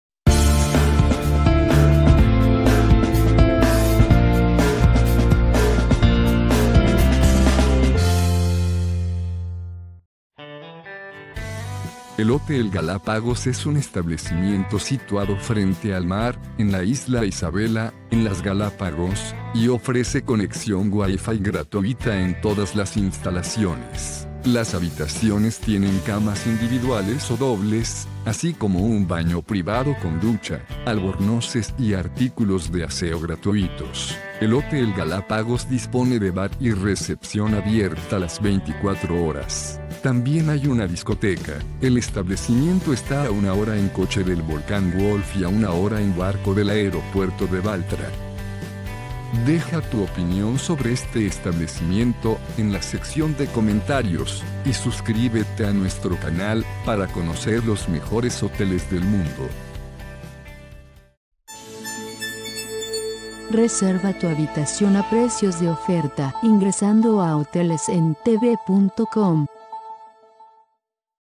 Hotel Galapagos en Puerto Villamil
Hotel Galapagos es un Hotel de nivel 4 estrellas ubicado en Puerto Villamil.